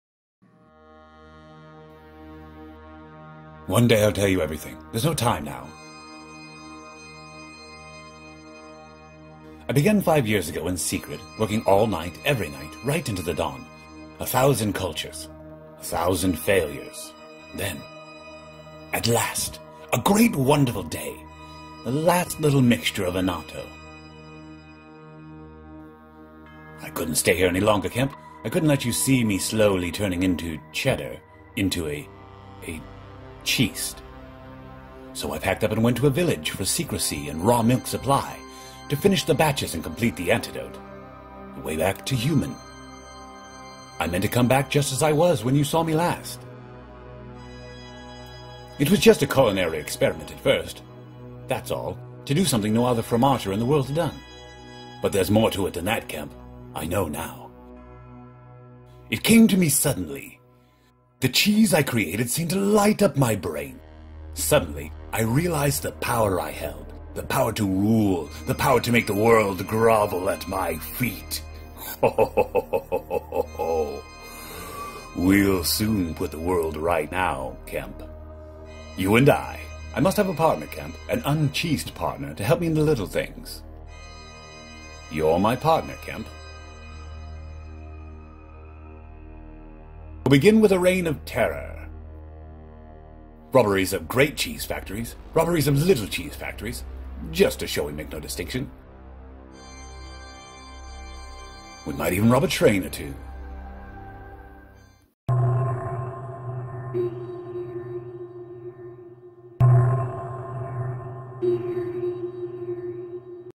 The Cheast Chronicles: "The Cheastable Girl!"
Wherein our Stalwart hero, beset upon by a Cheese Fiending Beast, known as the Cheast, realizes in horror the plans of the Cheast and his continued indentured life.  Will the Cheast Invasion, dear friend, be stopped before it consumes our world?!

Sequestration Day 35